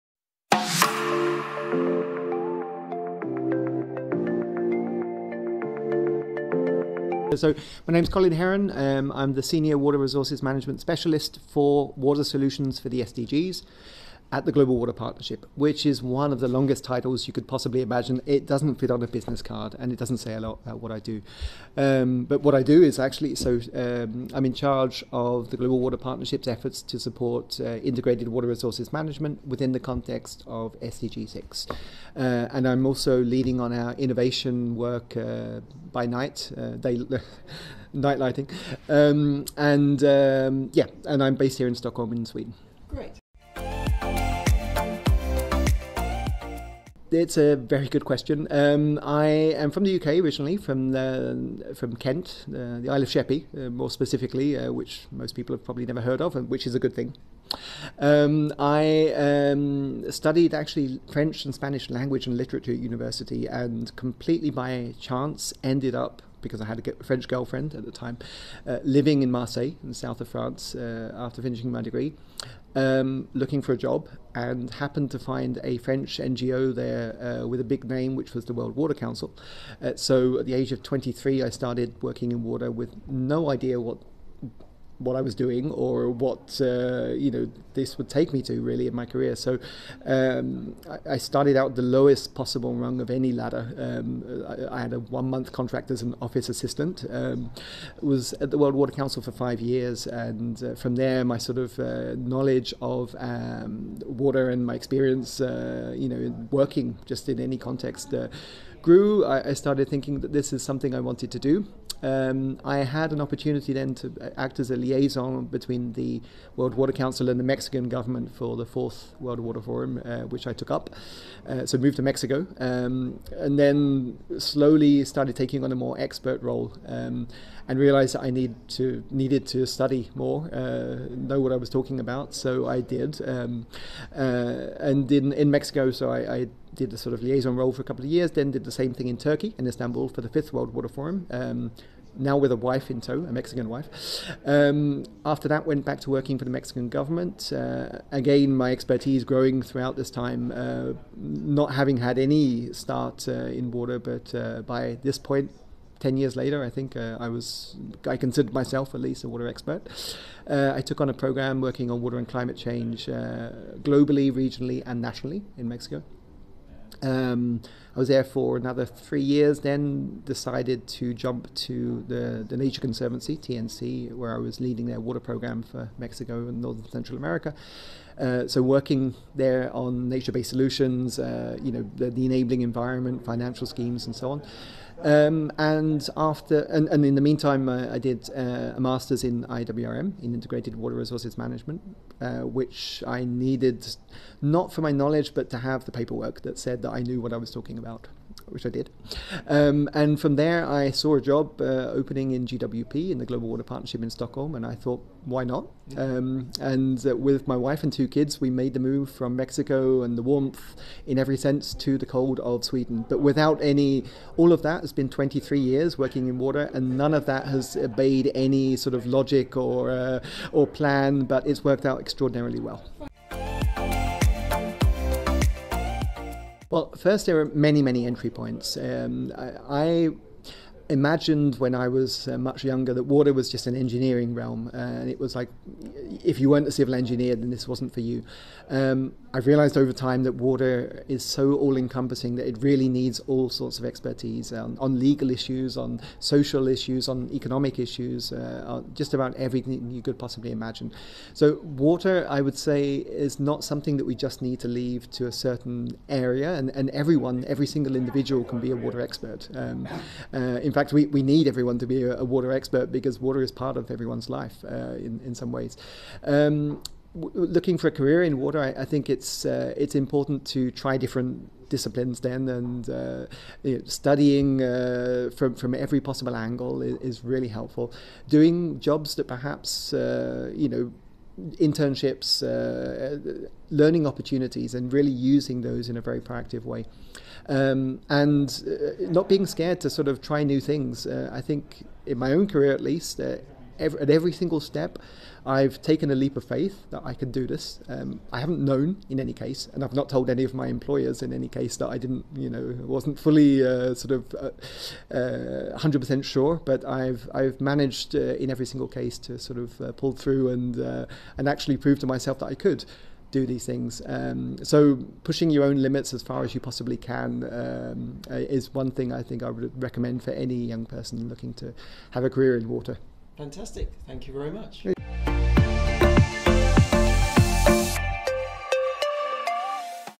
Career Chat: Colin Herron, Global Water Partnership
Interview with Colin Herron, Senior Water Resources Specialist, Global Water Partnership, Sweden, at UN Water Annual Meeting

Available now "How to guide the careers of young people in the water and sanitation sector"